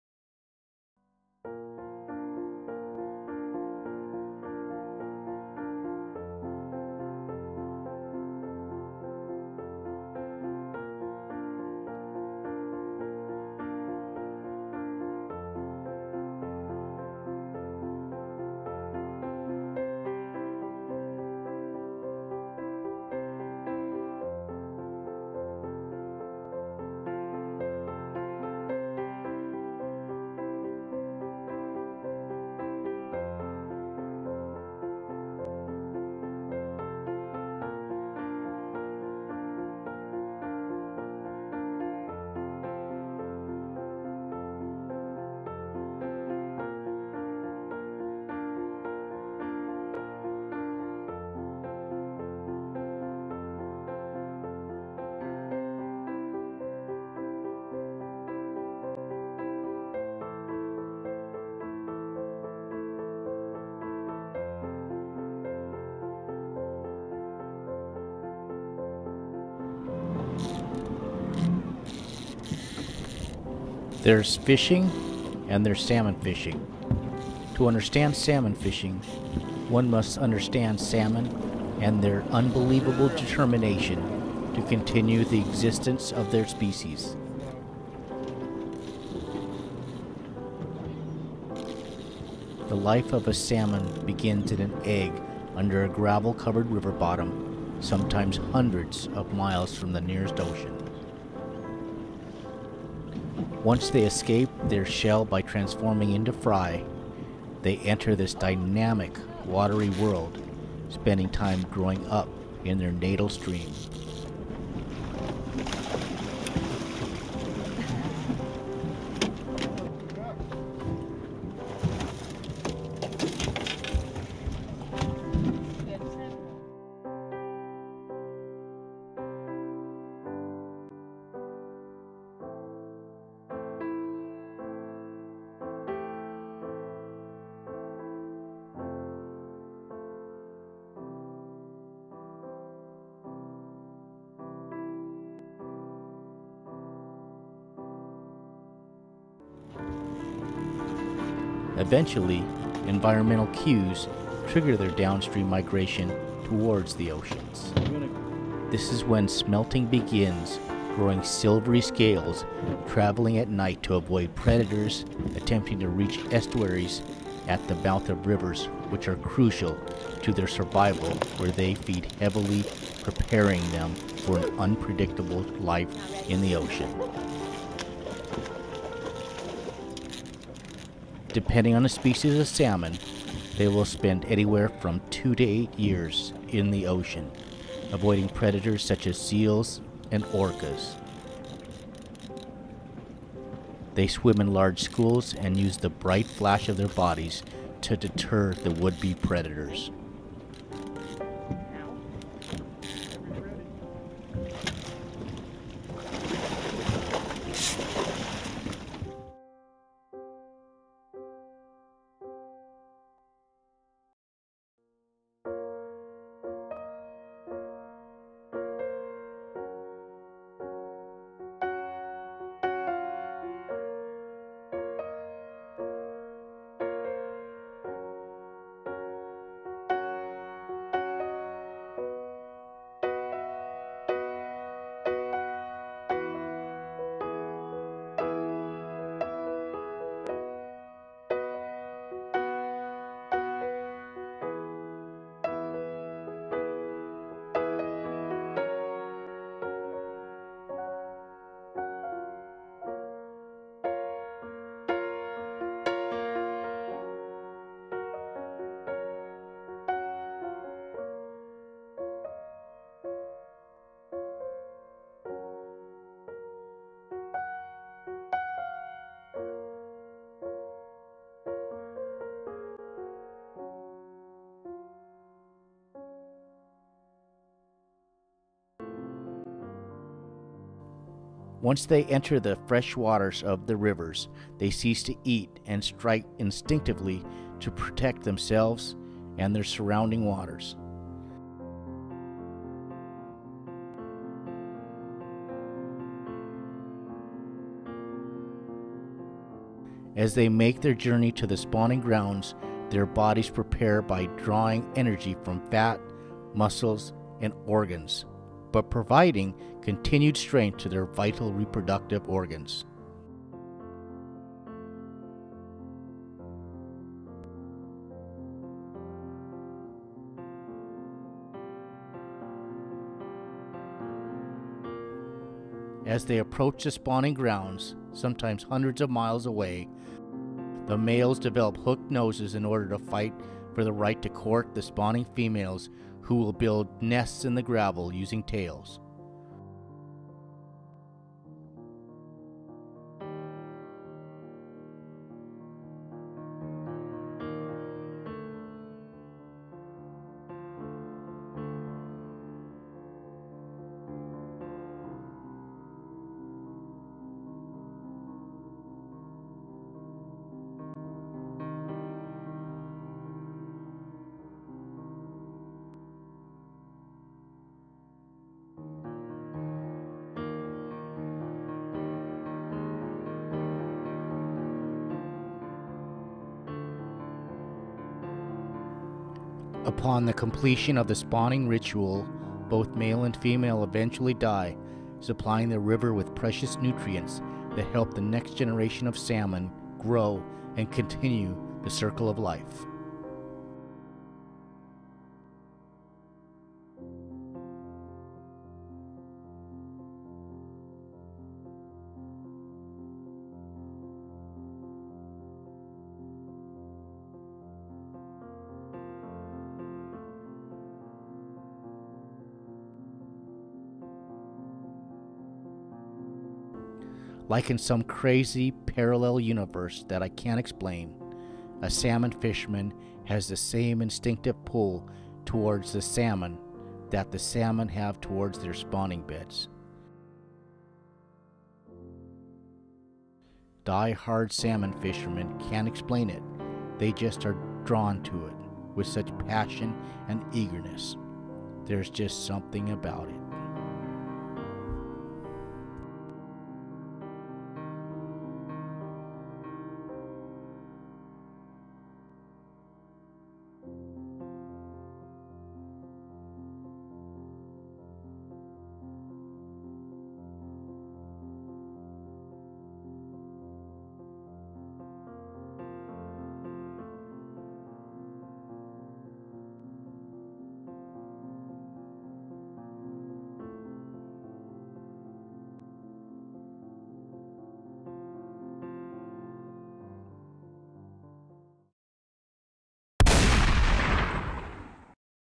The Wild 04 - Salmon Fishing
Salmon fishing in the Northwest's mouth of the Columbia River